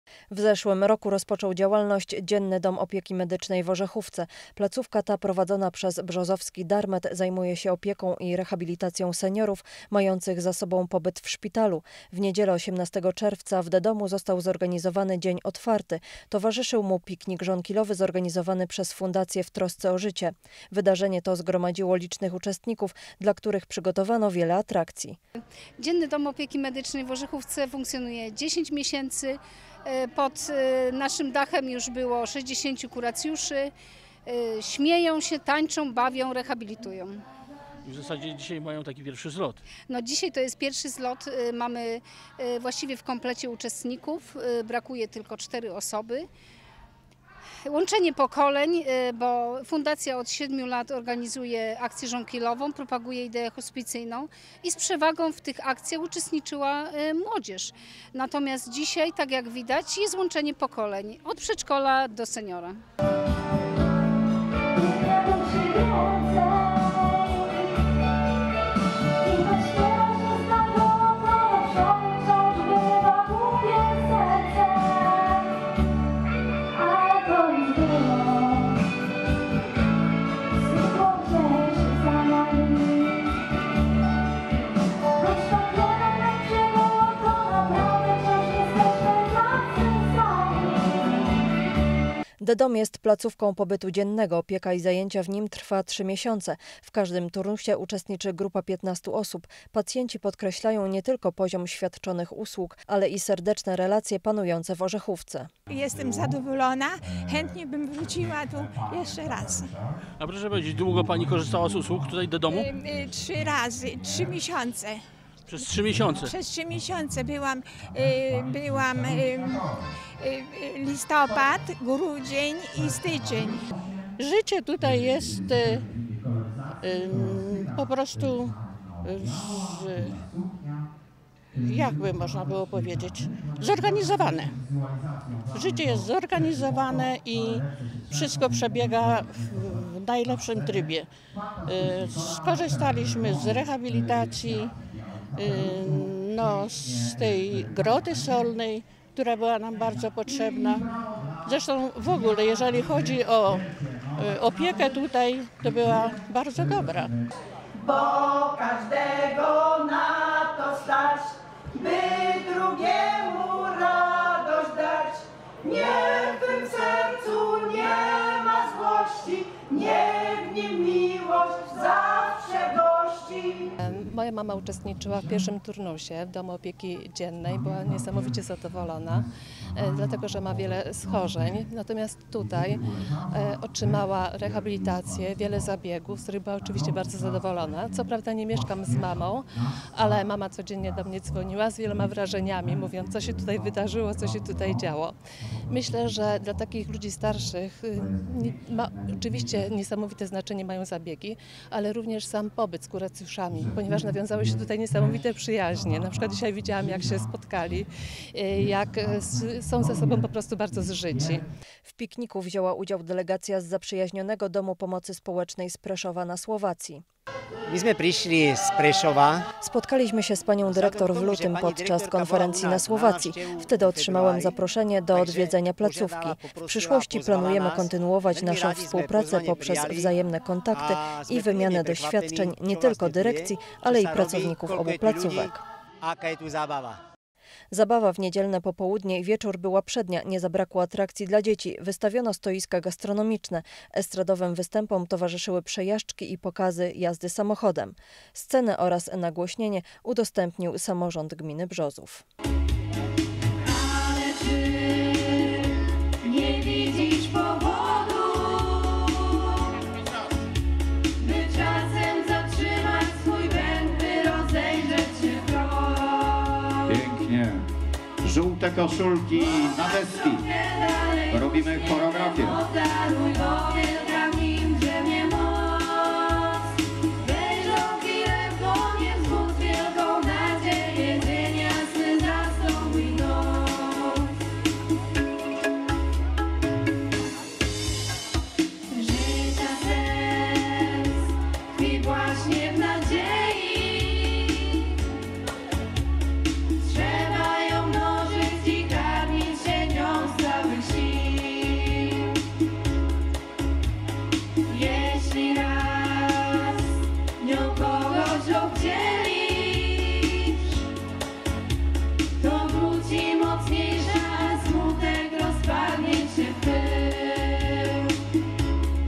Dzień otwary w DDOM-u w Orzchówce
W zeszłym roku rozpoczął działalność Dzienny Dom Opieki Medycznej w Orzechówce. Placówka ta prowadzona przez brzozowski Dar-Med zajmuje się opieką i rehabilitacją seniorów mających za sobą pobyt w szpitalu. W niedzielę 18 czerwca w DDOM-u został zorganizowany dzień otwarty. Towarzyszył mu Piknik Żonkilowy zorganizowany przez Fundację w Trosce o Życie.